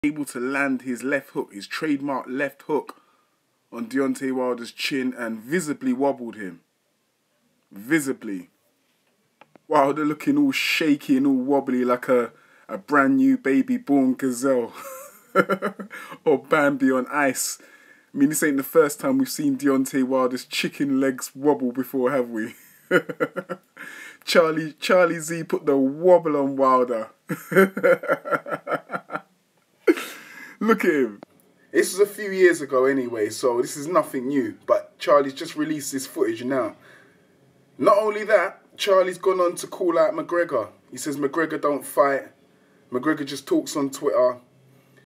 Charlie Z Beat Wilder! Visibly Landed and Knocked Wilder Out TKO Clean
Yeah I won that fight in the Ring, Mcgregor dont want the Smoke with Me since Im good enough to Beat Deontay that means im obviously too big and strong for that clown Conor